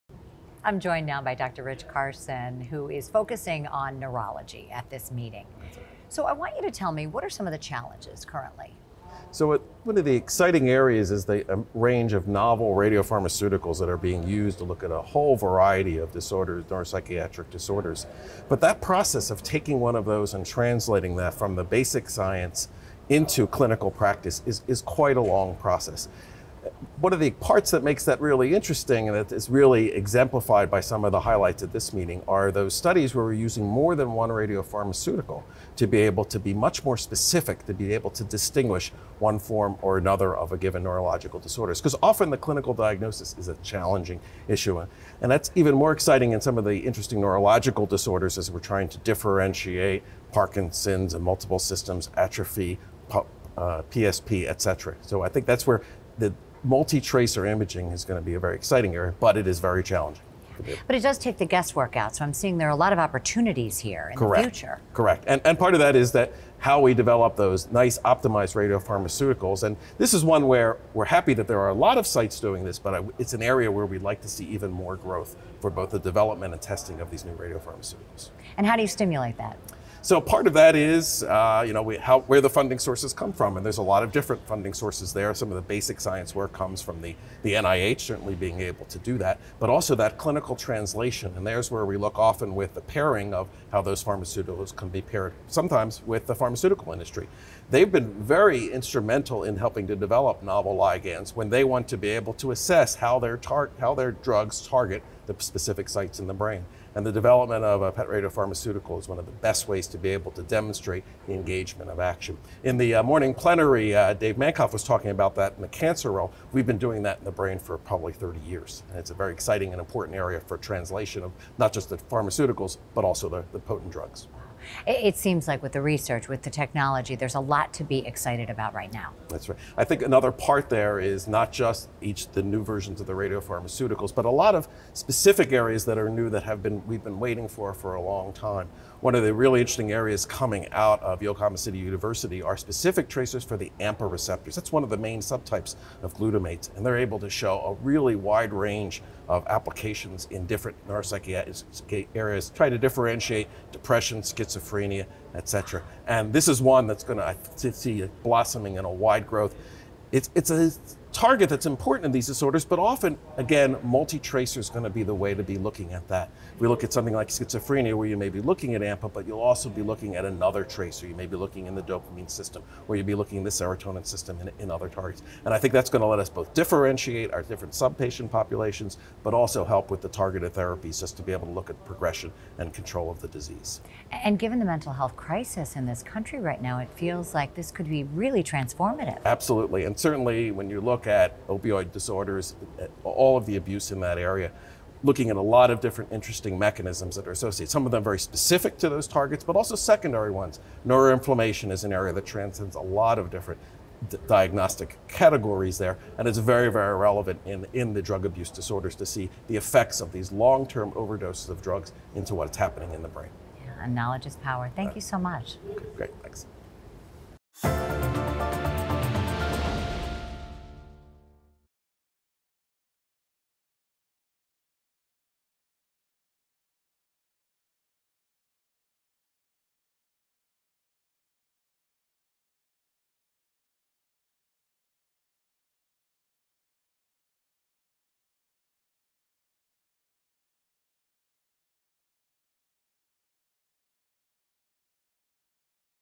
Neurology Highlights at SNMMI 2023 with Rich Carson, PhD
At the SNMMI 2023 Annual Meeting, SNMMI TV is in the studio with Rich Carson, PhD, the Professor of Biomedical Engineering and of Radiology and Biomedical Imaging from Yale University. He tells us all about the latest neurology highlights at the meeting.